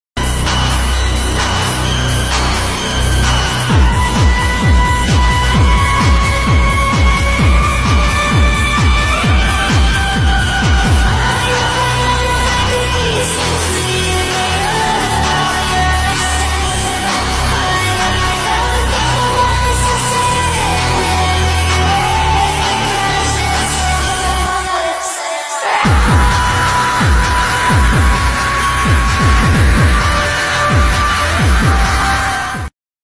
"I'm hungry..." || warnings: flash, shake || read des || ft.demon mitsuri || kny swap au
Explanation for video:
Basically, mitsuri used her blood demon art to hypothesise the guy with her voice. When mitsuri said "I'm hungry..." the guy who is hypothesized committed suicide for her

○warnings:
•Blood(I think)
•flash
•shake

♤Apps apps used:
~Gacha nox
~Ibispaint x
~Capcut